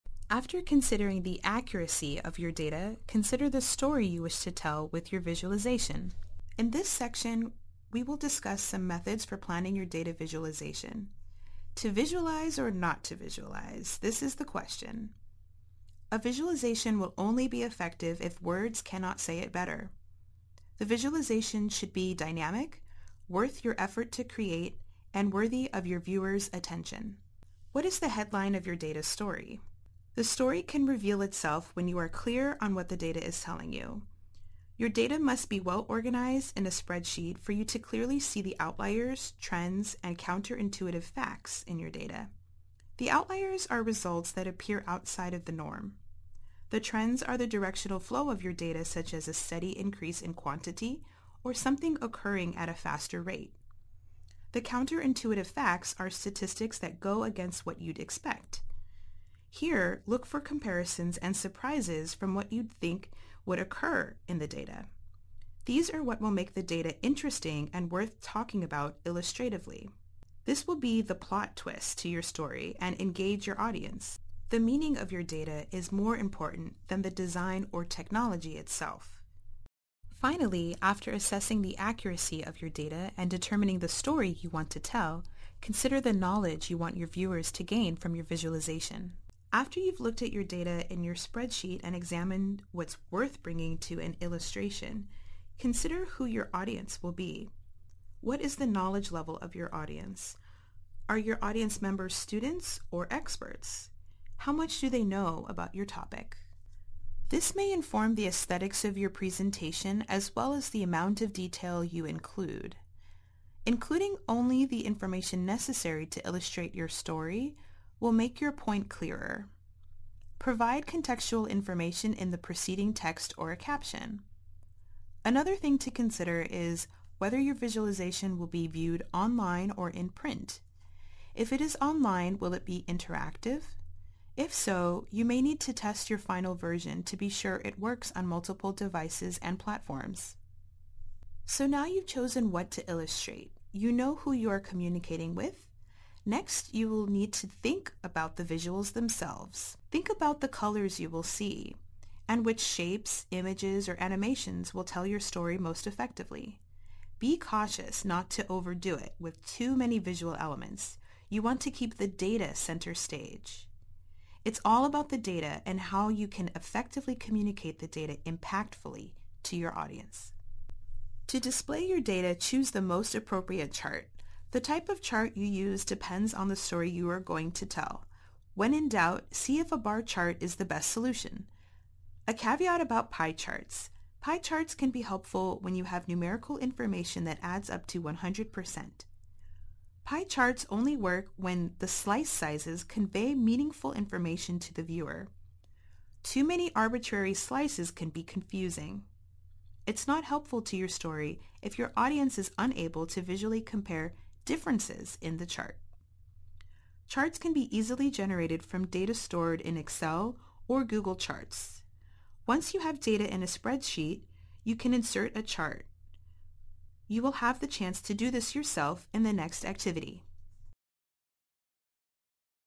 Module 2-7: Methods for Data Visualization
Watch this video to learn about methods for designing Data Visualizations.